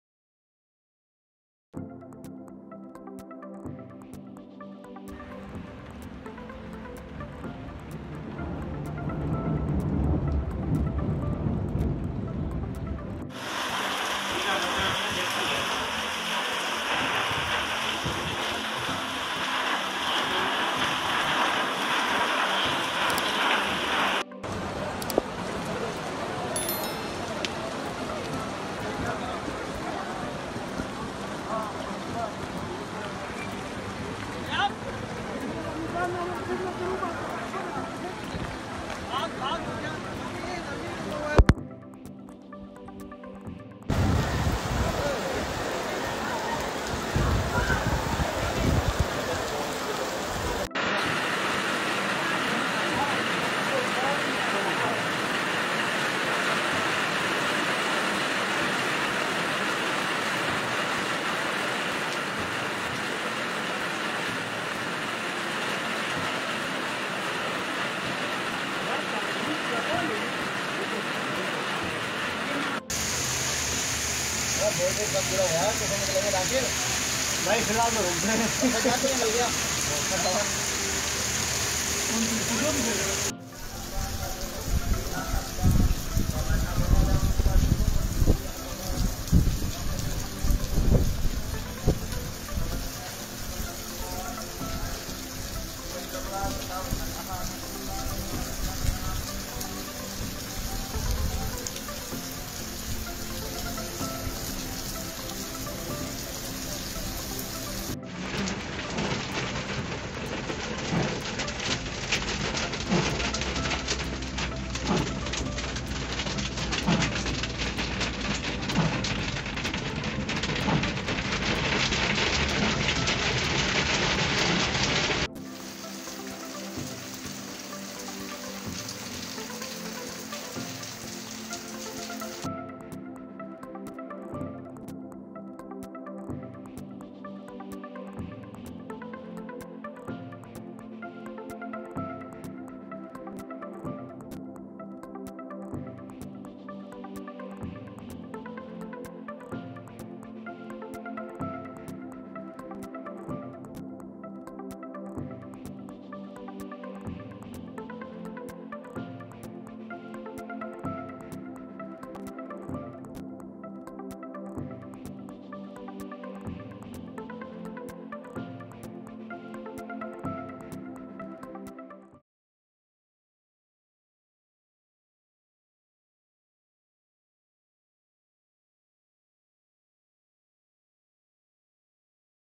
Heavy Rain in Karachi | Karachi Weather Updates News Today 2023 | Heavy Rain Fall in Bahria Town
Karachi Weather Updates News Today 2023

Heavy rains in Karachi and other parts of Sindh and Baluchistan have caused flooding. Here's a look at the rain in Bahria Town:

Heavy rains in Karachi, Sindh, and elsewhere in Pakistan. Check out our rain in Bahria Town video today for a look at the heavy rain in Karachi and other parts of Sindh and Balochistan.

Welcome to this mesmerizing video capturing the beauty of rain in some of the most scenic locations in Pakistan. In this video, we will take you on a journey through the lush green neighborhoods of Bahria Town, the serene and peaceful surroundings of Gaddap Town, the tranquil and picturesque landscapes of Kathor, and the vibrant and colorful streets of Gulshan Hadeed, as they get drenched in the refreshing rain.